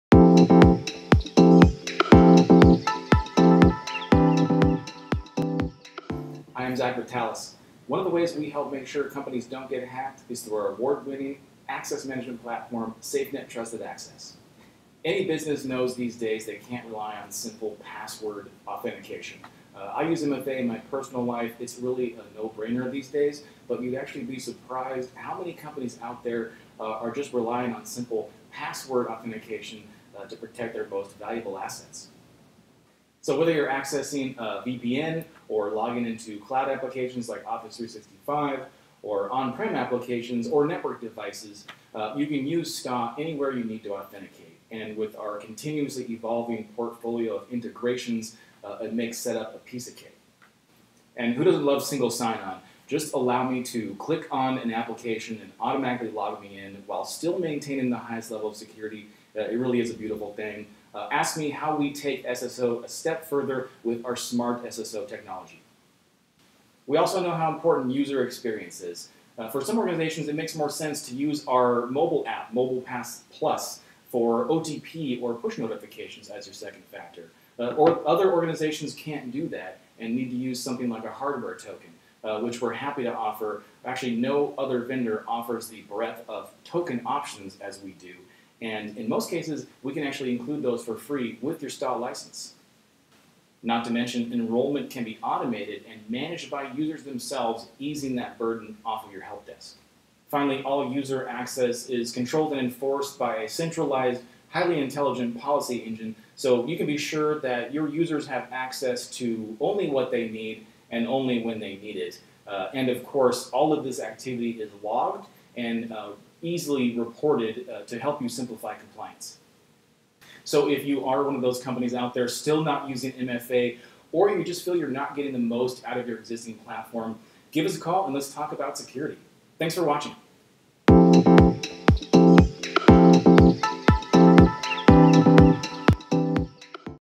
What is SafeNet Trusted Access?
SafeNet Trusted Access is an intuitive access management platform that makes it easy to manage user access to company assets and applications through Multi-factor Authentication, Single Sign-On and scenario-based policies.